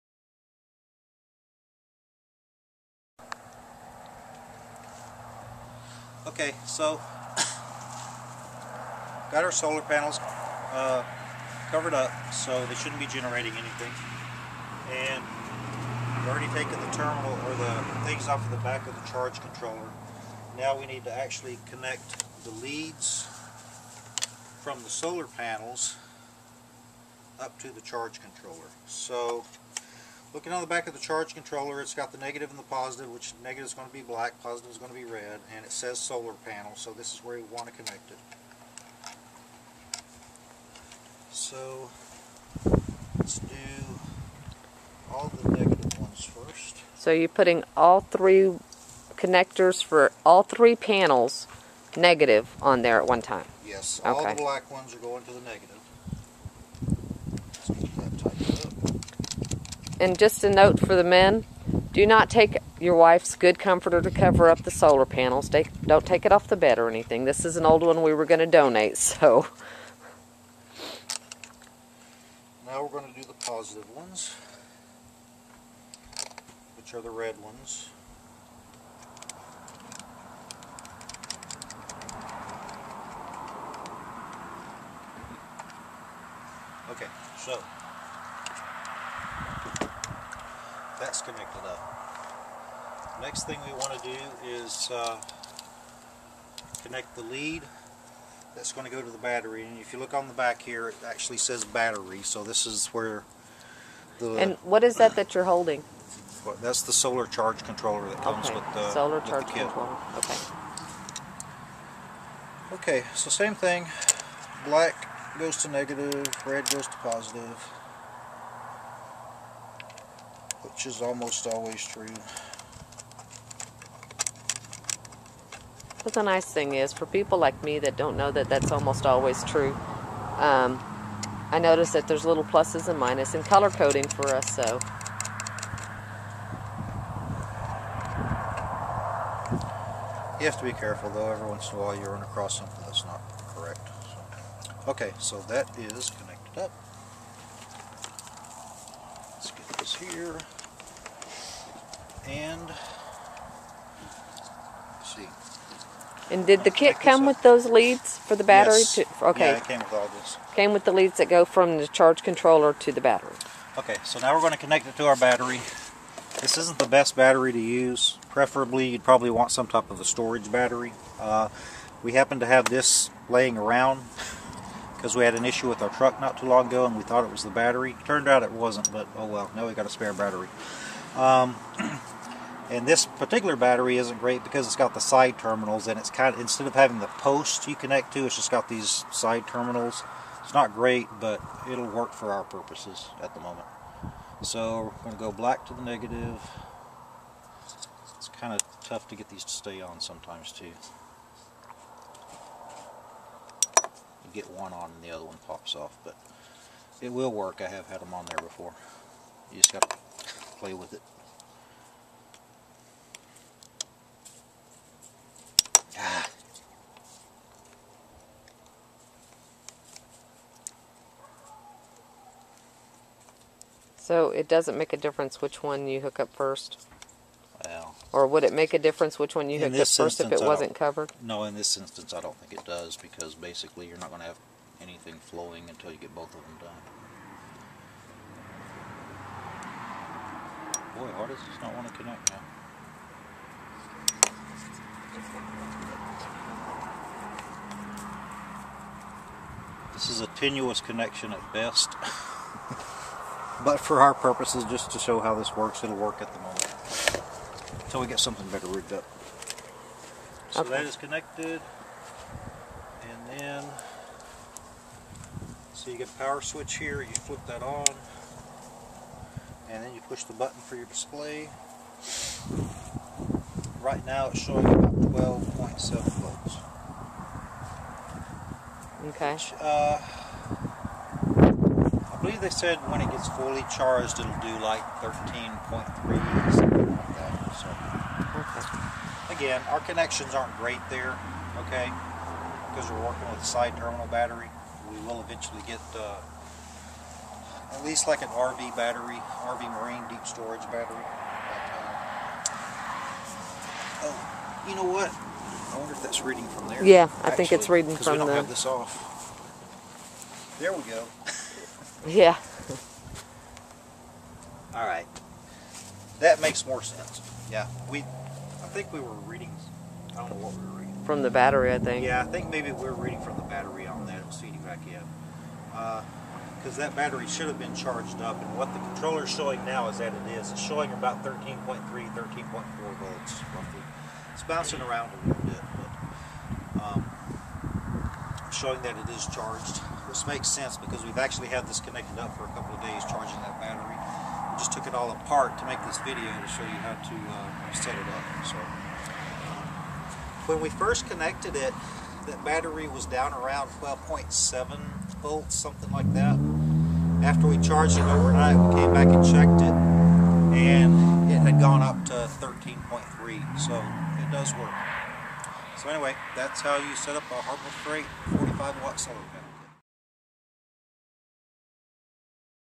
Testing our Harbor Freight 45 Watt Solar Panel Kit Pt 3
The last stage of the testing.  Well the last for now until we get a deep cycle battery.  When we get the correct battery we will hook up some items to see how well and how long they run.  For now the battery being charged will be our backup truck battery.